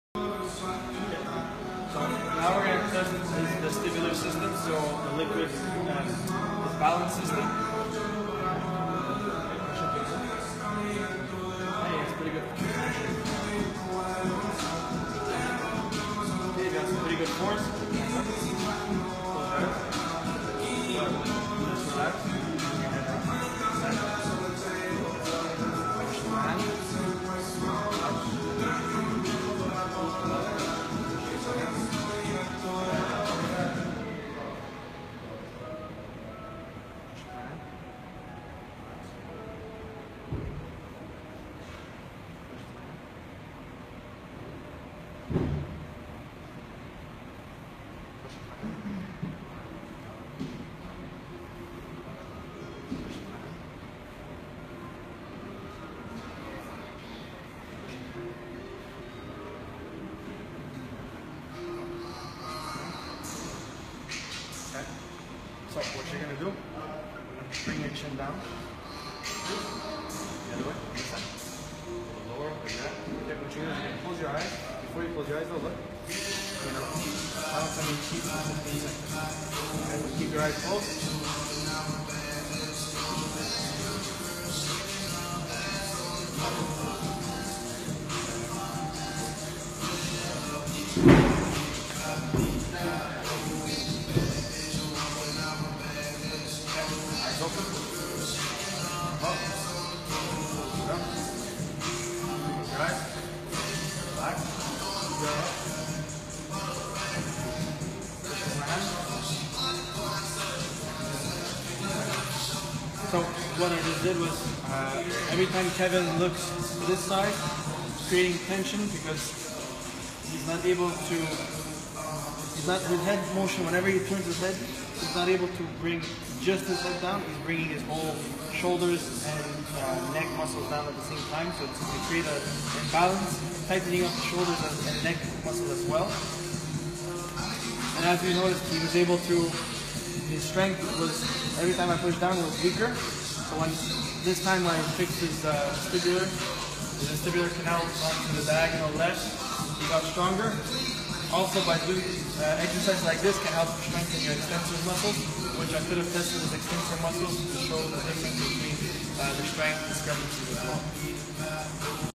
kevin nervous system correction
Correcting balance is one important aspect for developing athletes. If you correct the vestibular system, which vestibular system includes the parts of the inner ear and brain that process the sensory information involved with controlling balance and eye movements, it'll help correct the balance and tension in shoulders as well as back.